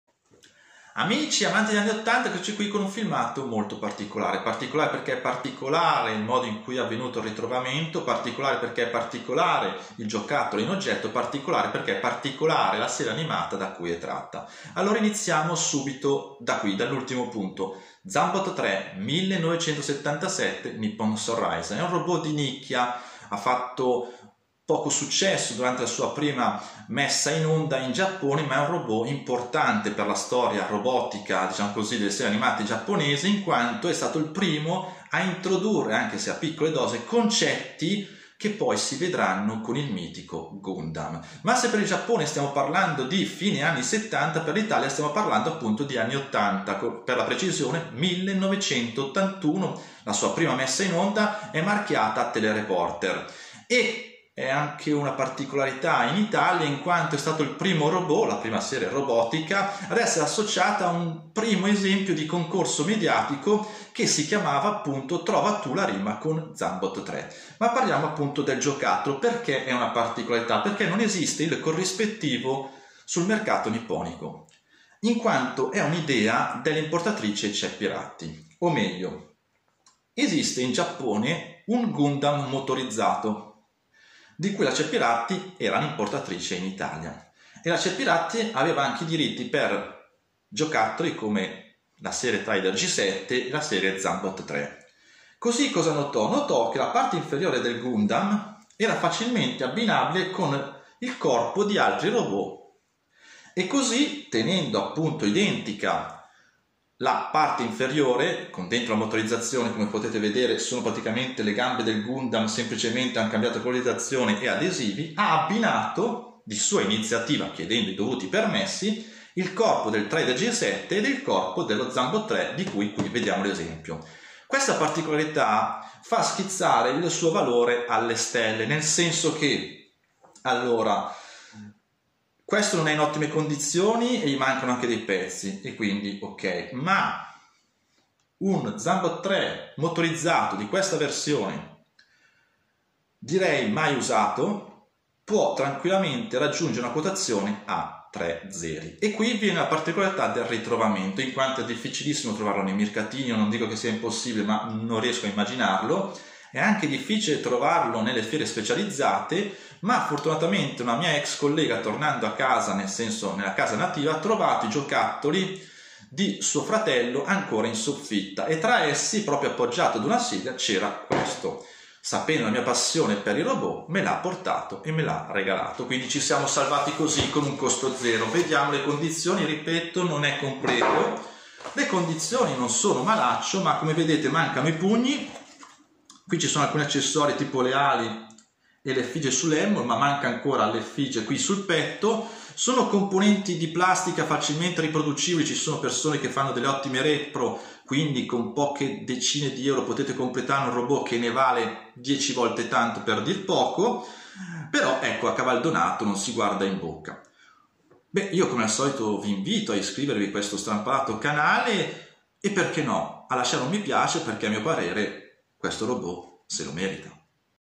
Zambot 3 (Zanbot 3) Battery Operated (Ceppiratti) - Robot Motorizzato
Uno Zambot 3 (Zanbot 3) particolare pieno di curiosità e di inventiva made in Italy.
E voi ... cosa ne pensate?